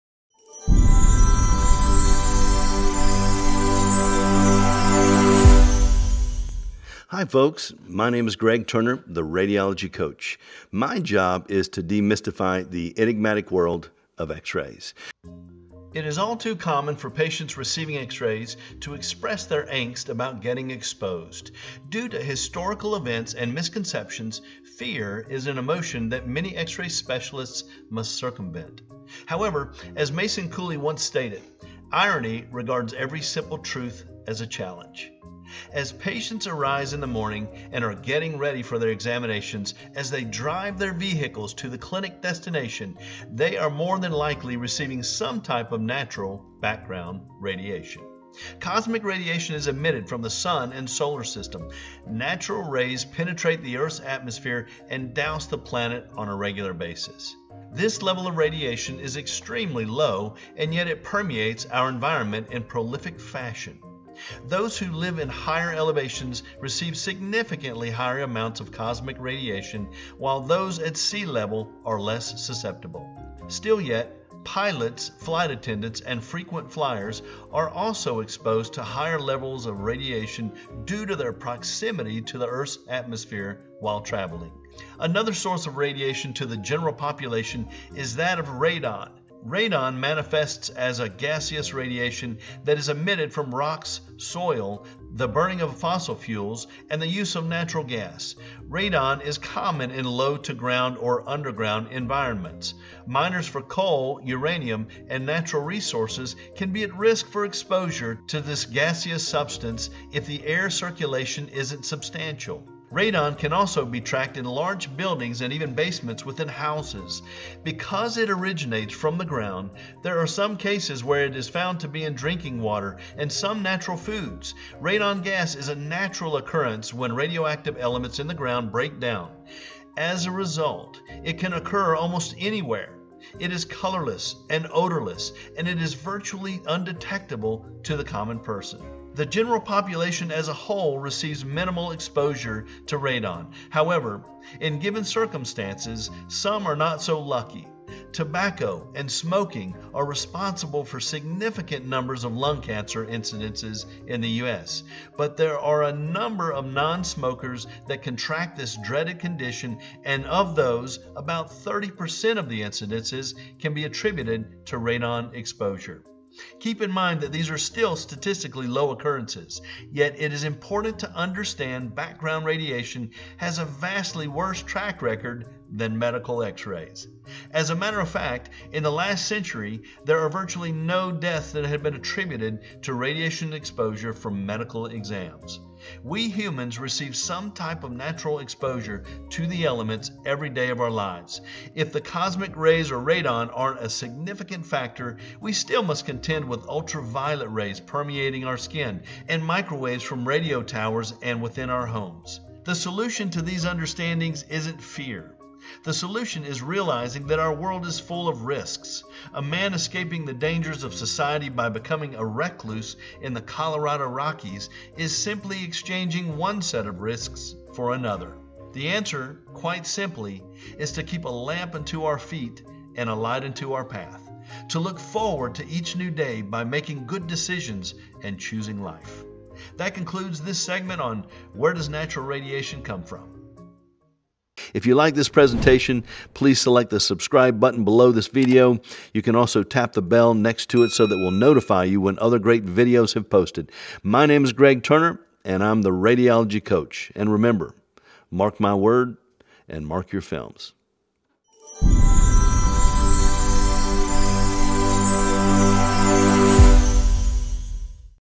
WHERE Does NATURAL RADIATION Come From??
The general public is no stranger to radiation exposure. We encounter it on a daily basis. The Radiology Coach discusses the various sources of background radiation and how they affect us.